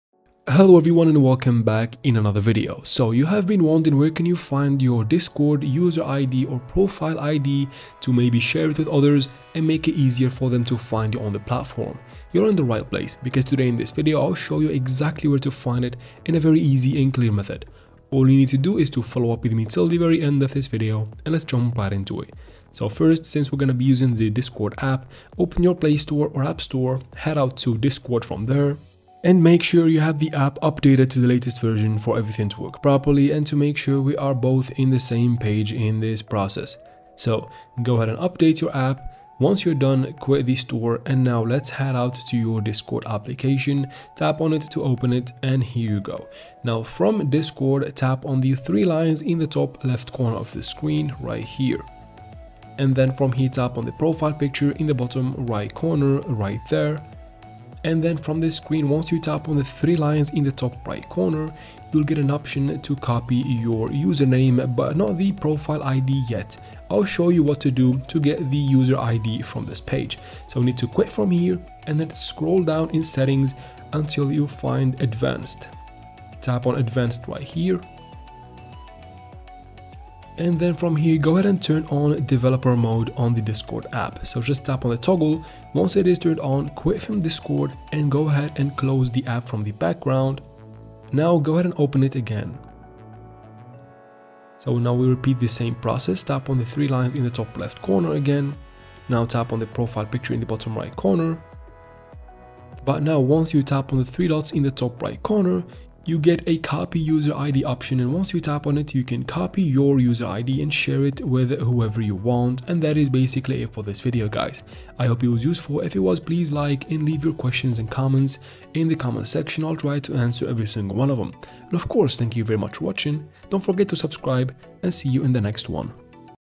How to Find User ID on Discord Mobile (2024) - IOS/Android
Do you want to know how to find user id on discord mobile? Then this is the video for you! I will show you how to find your user id on discord mobile.

This is a step-by-step tutorial on how to find discord user id on mobile in 2024. You can learn how to get discord id on mobile, so don't worry.